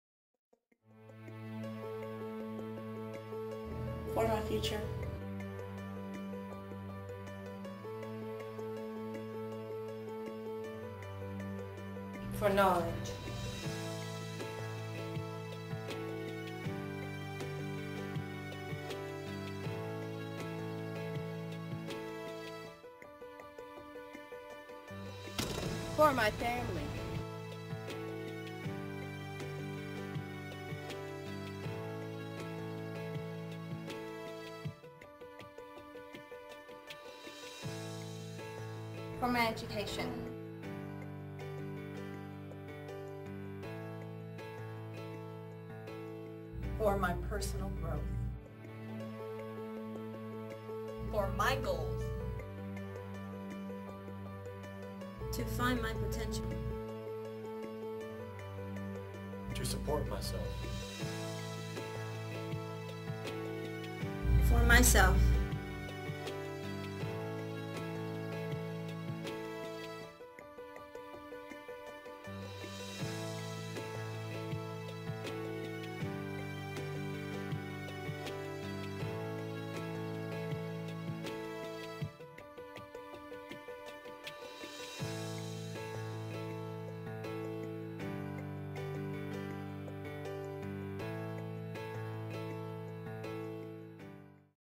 TSTC Commercial
TSTC commercial created by Brownwood, Texas students. Reasons to attend college.